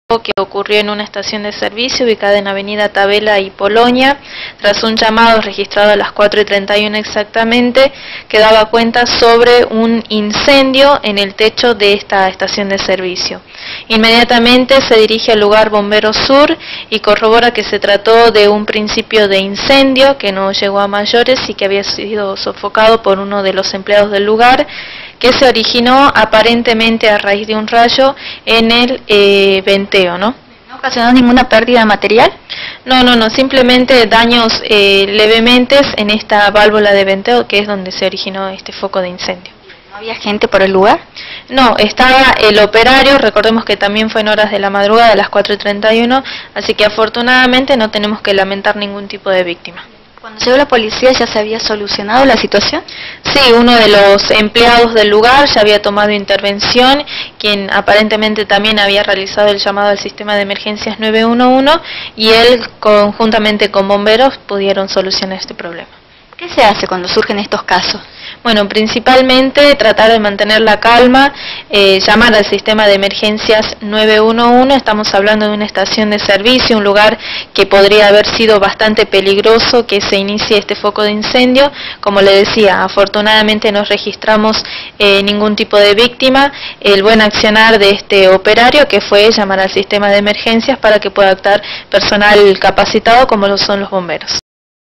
Cayó un rayo en una estación de servicio
Programa:C4Noticias
Alejandra Ale, Prensa de la Policía
La caída del rayo sólo provoco un principio de incendio que pudo ser controlado